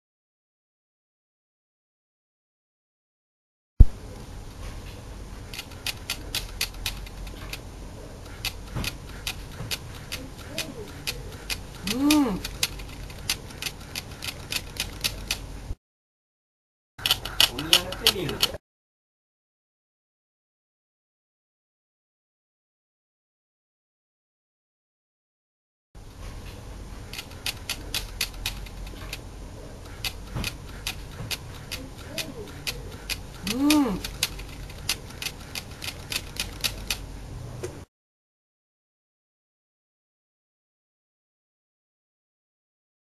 Toy Story - What they do when you are away
Toy Story - What they do when you are away, A story of a Elephant and a horse. Camera by the horse rider. Toy Owner. present i must say.. its Tre.